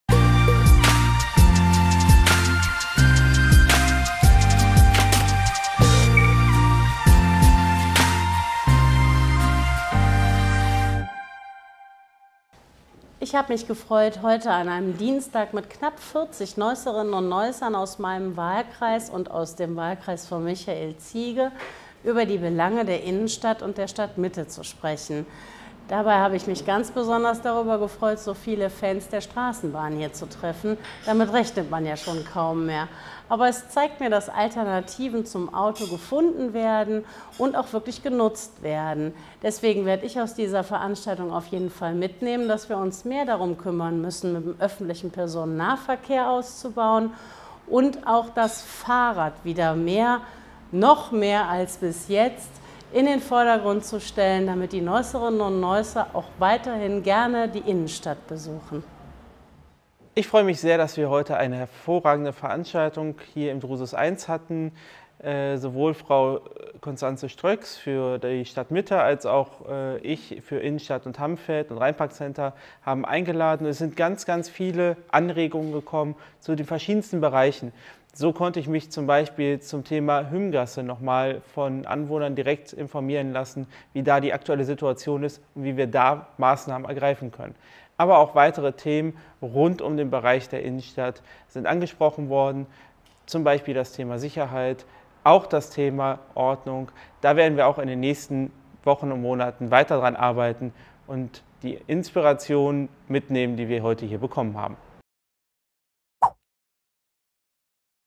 Auf ein Bier mir der Neusser SPD im Drusus 1
Stadtratskandidaten Michael Ziege und Constanze Stroeks am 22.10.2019 beim Bürgerdialog im Drusus1 in Neuss.